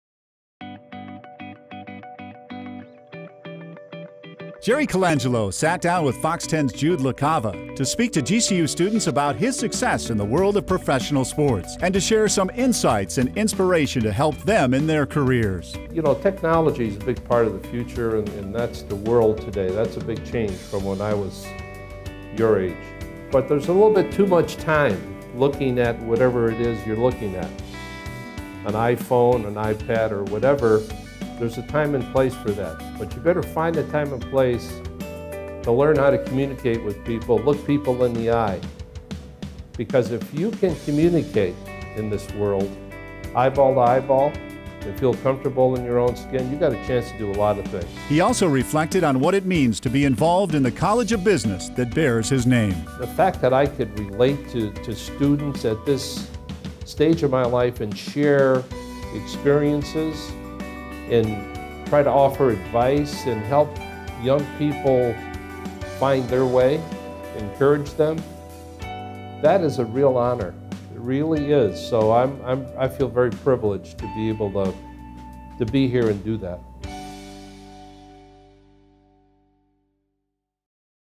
Jerry Colangelo Shares Stories of Success
Jerry Colangelo, business icon and the namesake to the Colangelo College of Business sat down with Fox 10's Jude LaCava to give us his stories of success in pro sports and in business.